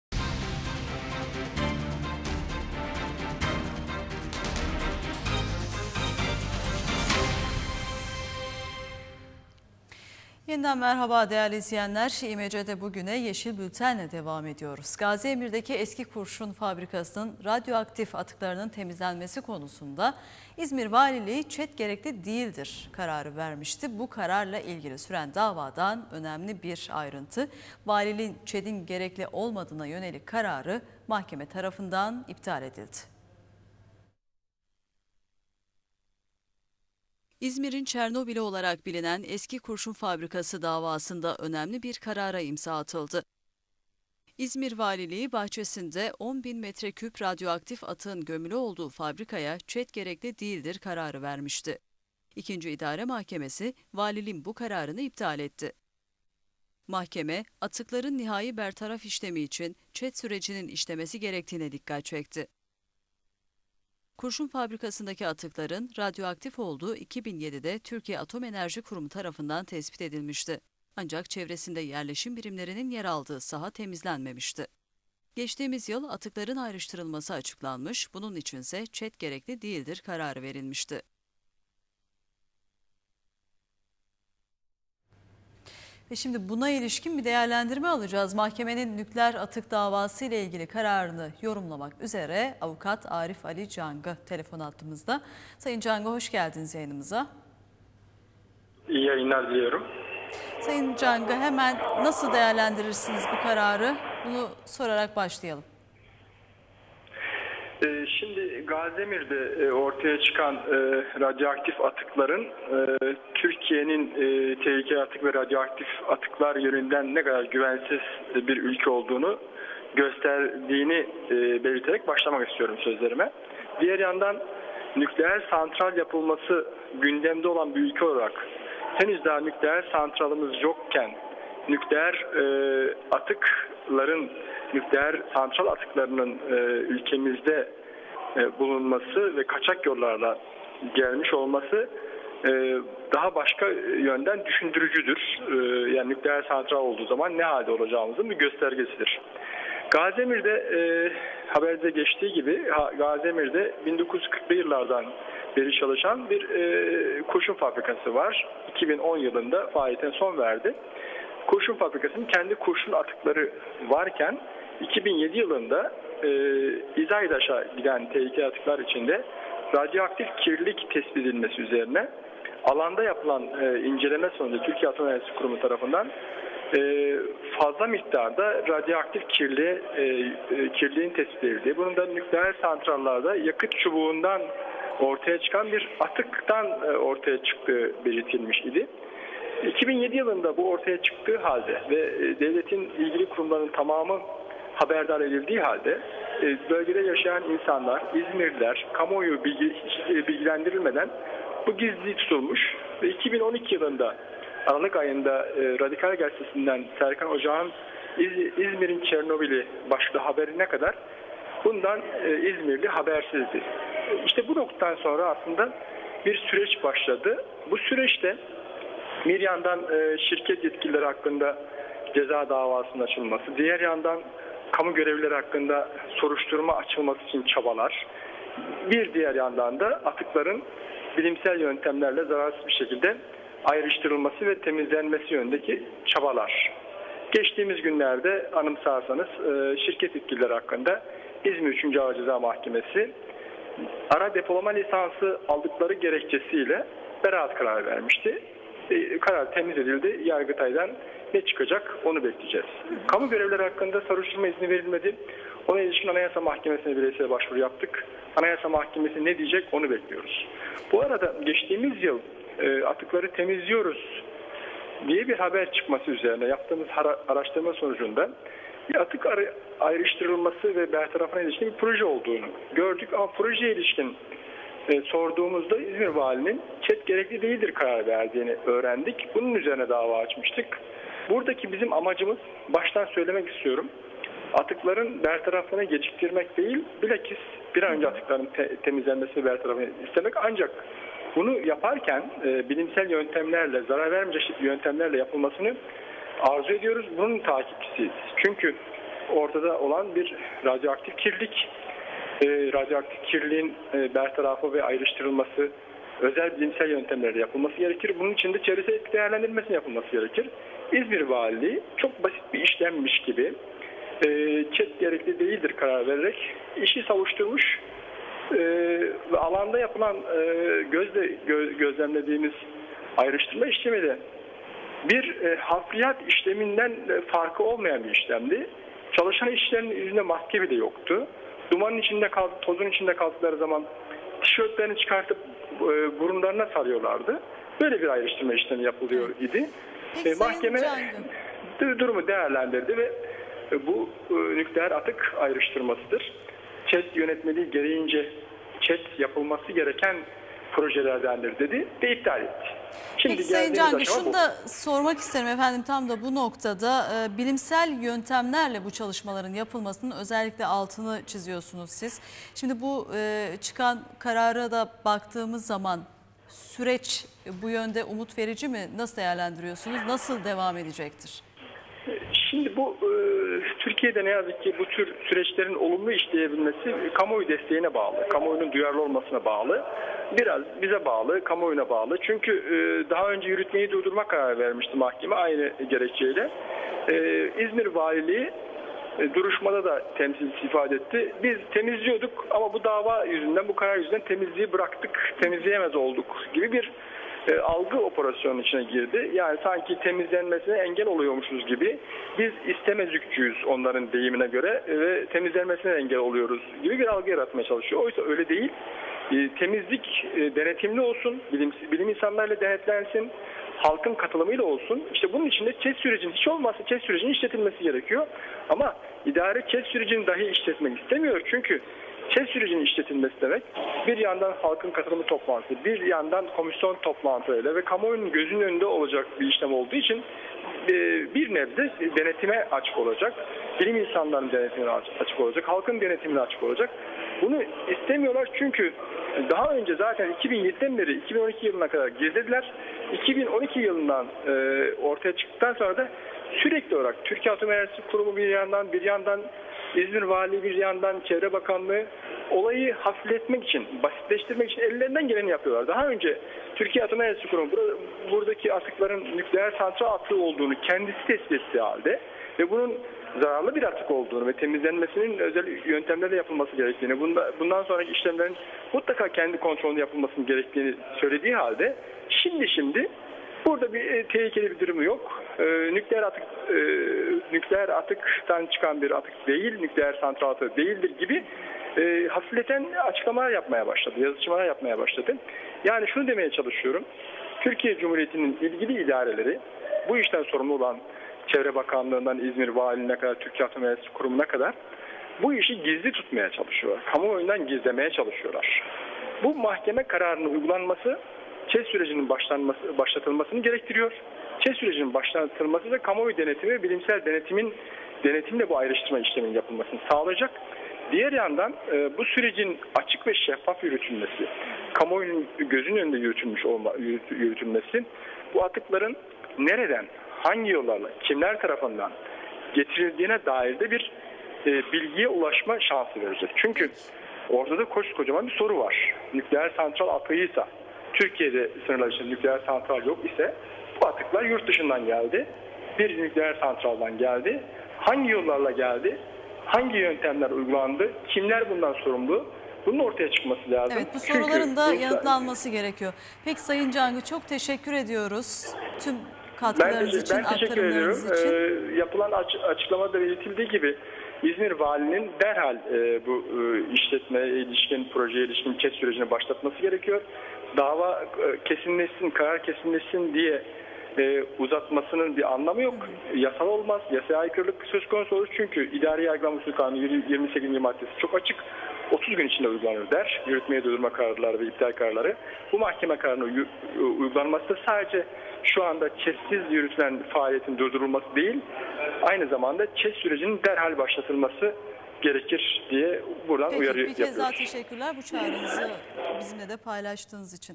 İzmir'in Çernobil'i davasında önemli karar
İzmir Gaziemir'deki nükleer atık davasında önemli bir karara imza atıldı. Atıkların ayrıştırılması için verilen "ÇED Gerekli Değildir" kararı iptal edildi. Kararı avukat Arif Ali Cangı yorumladı